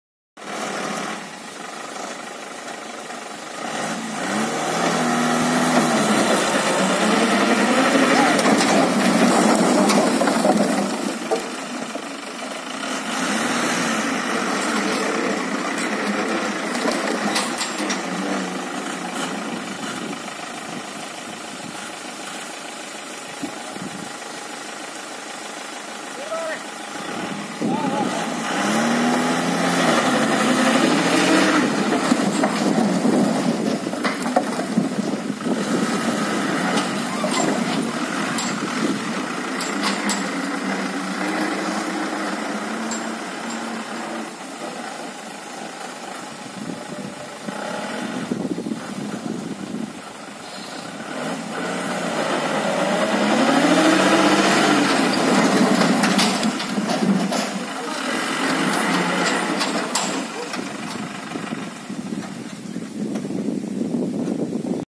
ഒരു മിനിറ്റ് മതി ഒരു ലോഡ് വിറക് ഇറക്കാൻ
Unloading firewood with in one minutes, Tata407 viral vedio